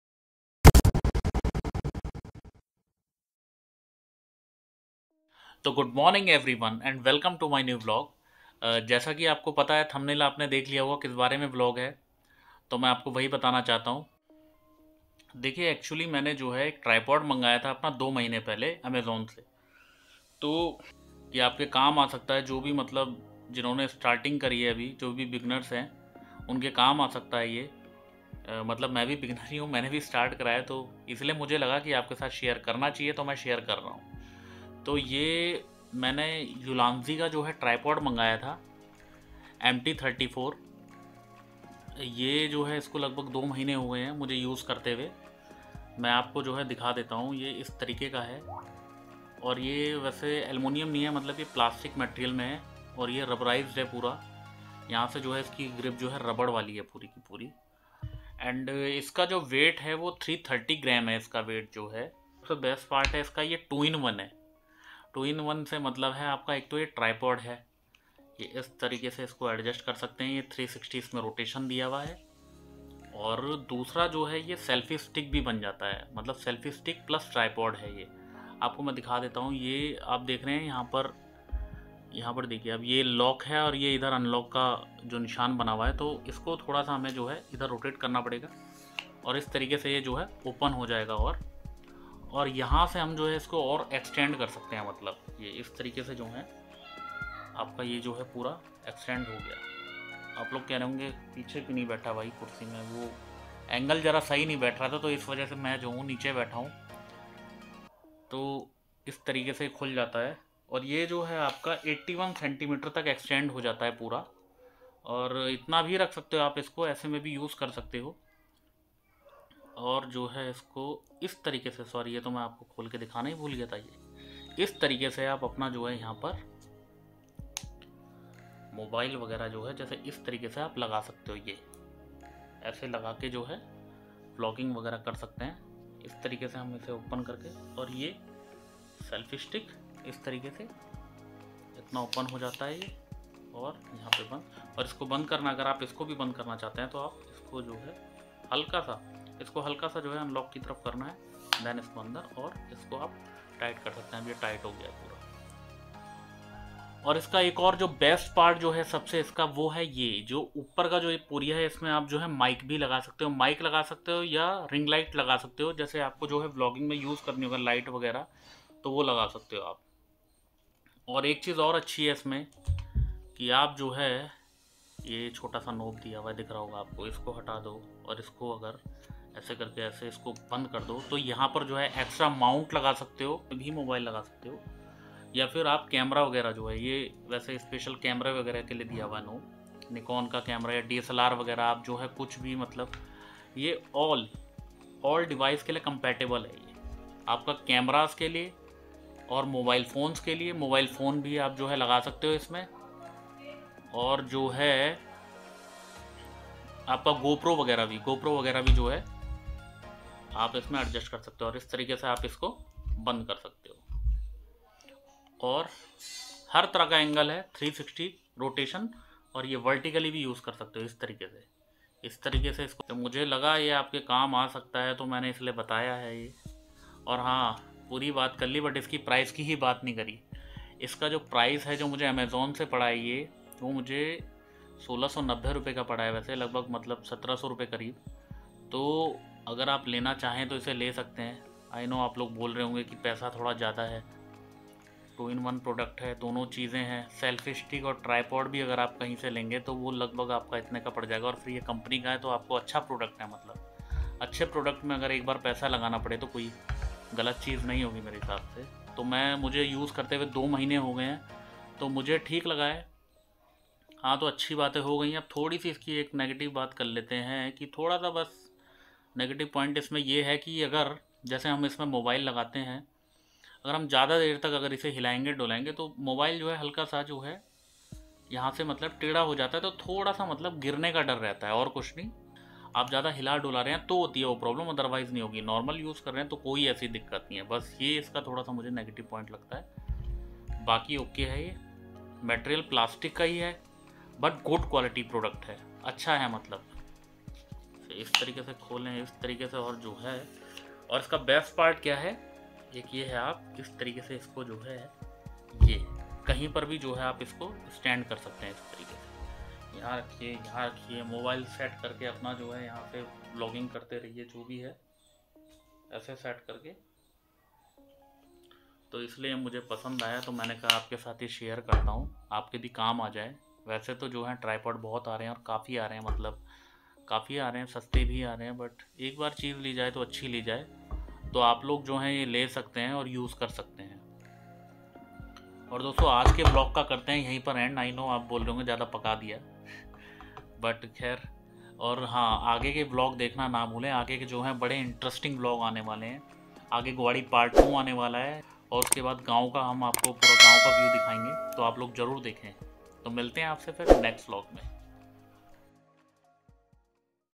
Ulanzi MT -34 tripod |Best tripod for Mobile phone | Tripod and selfie stick| Tripod for vlogging
Hey guys Today i will show Best Ulanzi  MT-34 tripod  For DSLR & Smartphones and  compatible with all devices .Ulanzi MT-34  this tripod is serioulsy Amazing at this price range and i hope you guys like it...and please do like share and subscribe  and don't forget press the bell icon.

best tripod#
# ulanzi#
Best tripod for mobile phones#